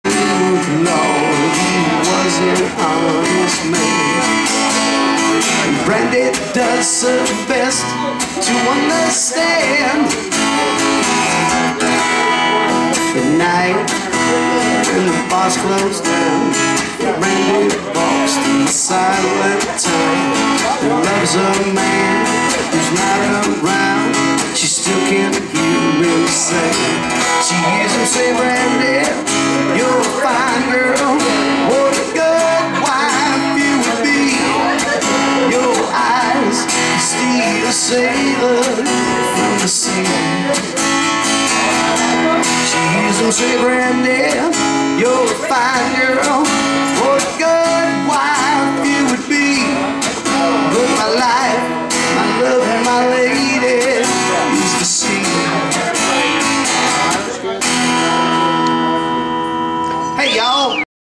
Ray Coustic & Hollywood's Cycles Bike Night
RaY'coustic performing at Hollywood's Cycles 2010 Bike Night at The Sandtrap Bar & Grill on Westinghouse.

Video by:  LL's Magic Lenz